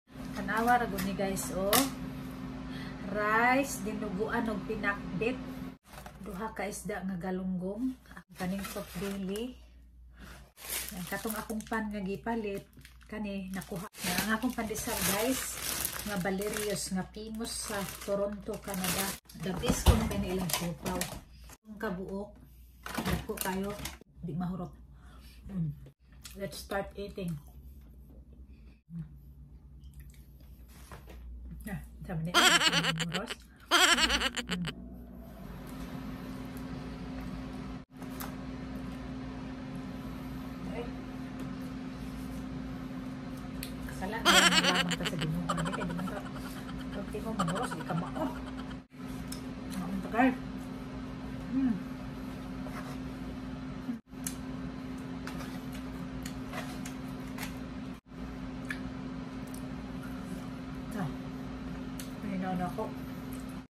Dinner time/Dinuguan, Pinakbet, & Pork Belly
Ang sarap ng dinner ko. Satisfied my cravings pinoy food.